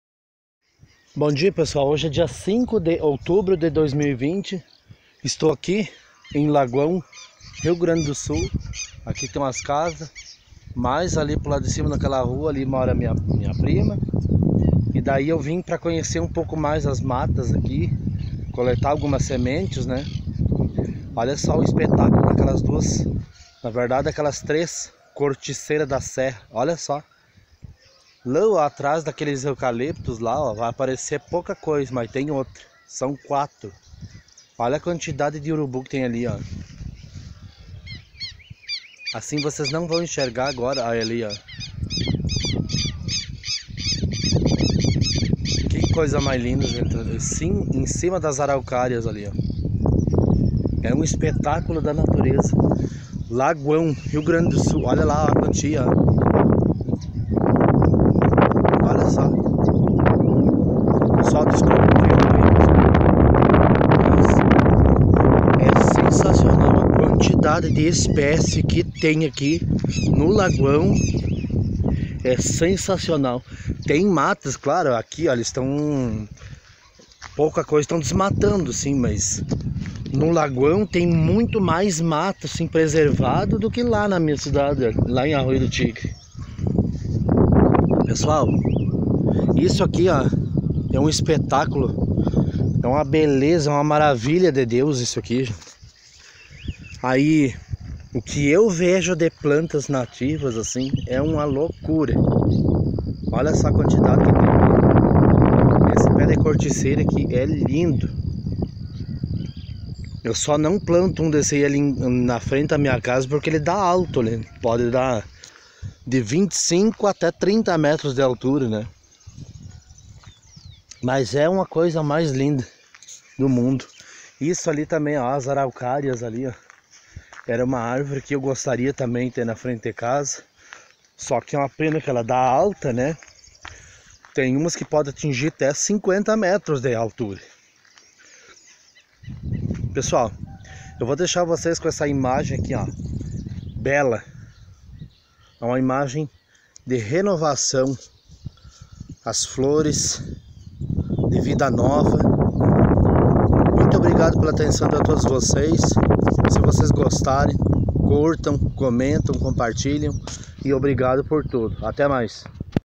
Belezas naturais do Lagoão(RS)
Dia 05 de outubro de 2020(segunda-feira)  vim mostrar para vcs um pouco das belezas naturais do Lagoão(RS). As majestosas corticeiras da serra com suas bela florada.